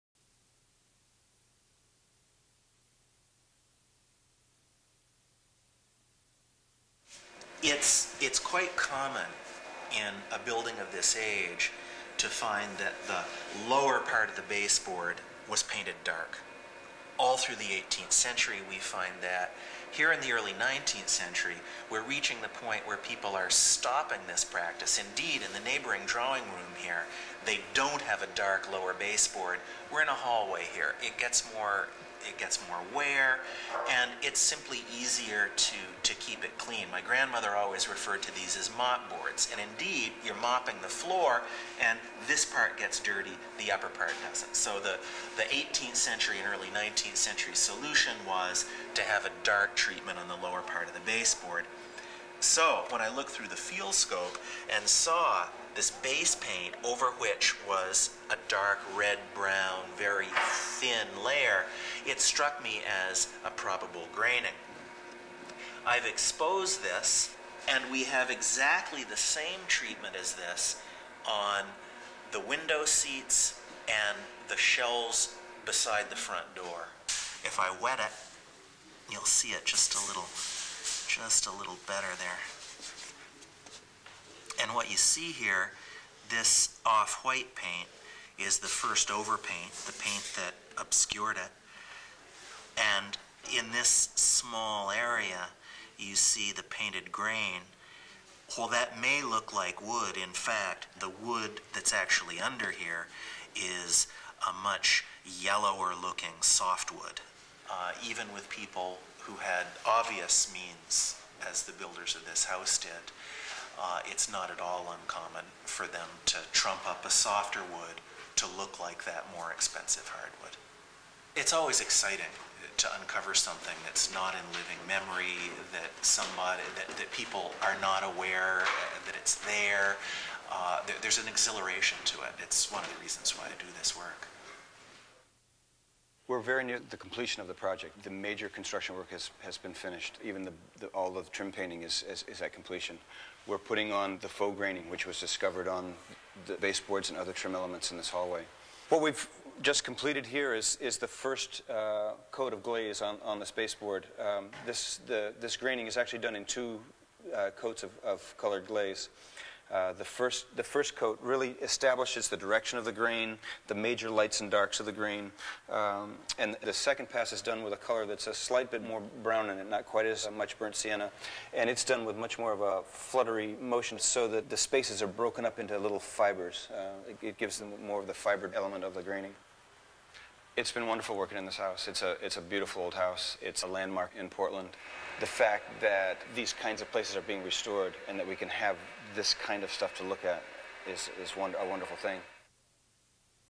Faux Mahogany.mp4
One of the restoration stories of the McLellan House at the Portland Museum of Art.